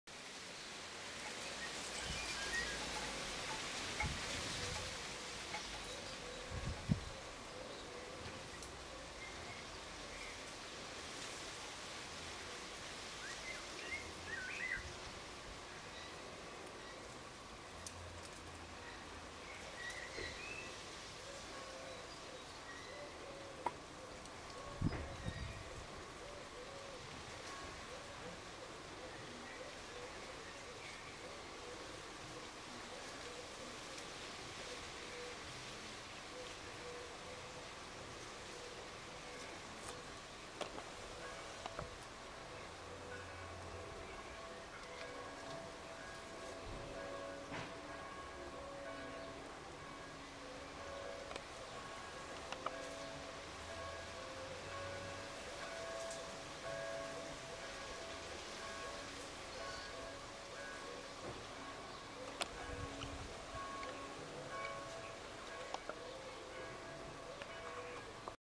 Siep en Guus, lazy afternoon in our backyard :P
Our 2 gorgious tomcats (both 9 months old at that time) found a nice place in the afternoon-sunlight.

Life is good :D